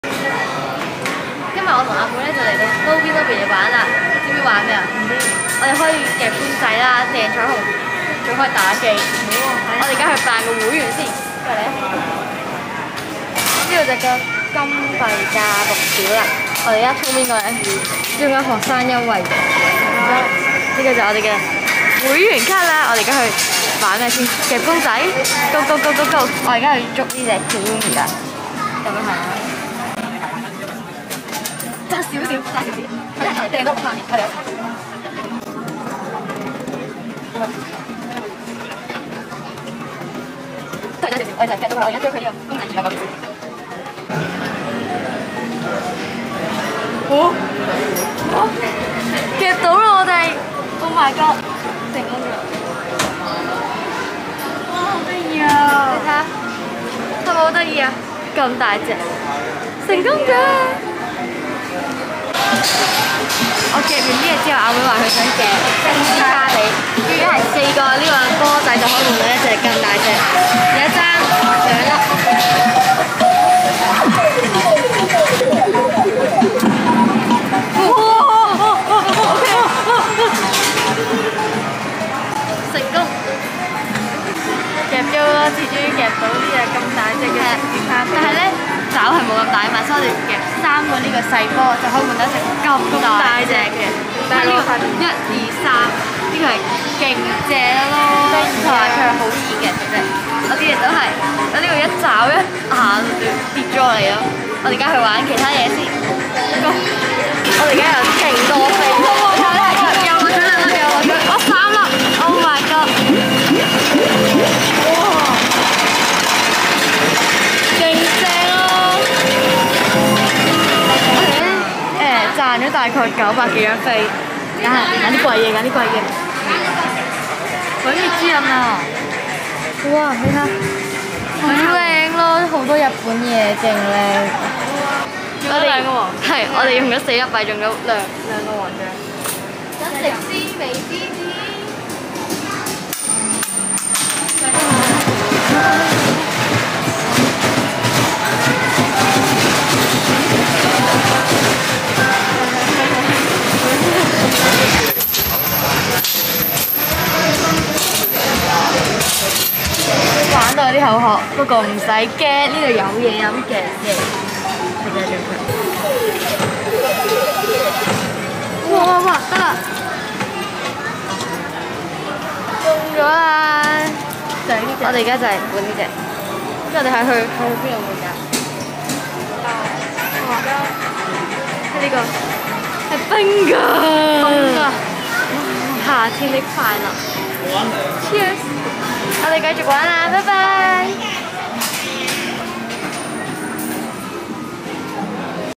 青衣城Nobi Nobi夢幻樂園夾公仔！
有夾公仔區🐻、幸運彩虹🌈、遊戲機區👾攤位天地同兒童室內遊戲場🎡，仲可以用🎟獎票換禮物添😍D禮物仲超級靚🎁
兒童室內遊戲場🎡啱哂小朋友放電！仲特設全日票，一個價錢就可以玩足全日！
💰代幣大約一蚊一個，增值越多送嘅金幣都越多～俾$10就可以成為會員換禮物啦！